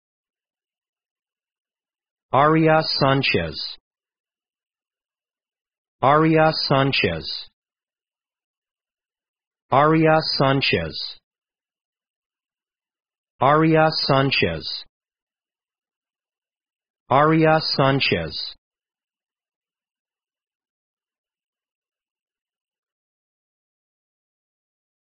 How to pronounce Arias Sánchez
Arias Sánchez American English pronunciation. How to pronounce Arias Sánchez correctly. How to say Arias Sánchez in proper American English.